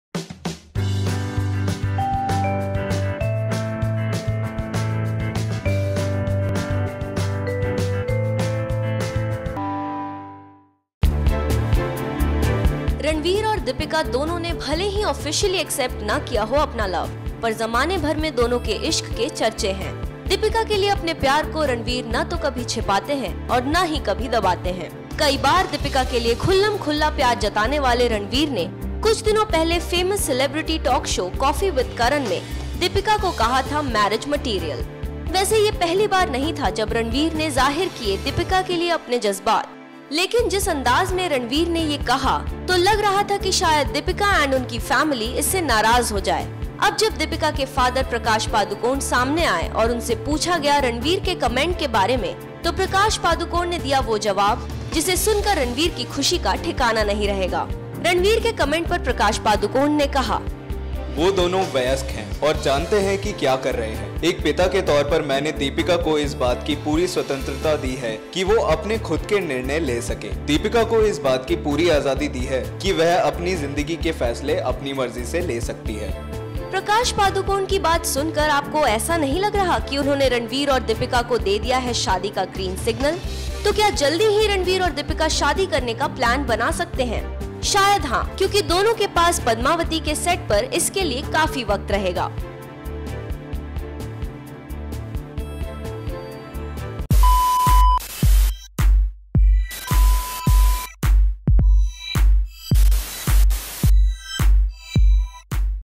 Ranveer Singh को Deepika Padukone के पापा से मिल गयी शादी की Permission
When Ranveer Singh was recently asked about Deepika Padukone on a popular chat show, he stated that he'd like to marry her as she is "marriage material".Now, the actress' father Prakash Padukone opened up on the subject  said that both Ranveer and Deepika are adults and know what they are doing.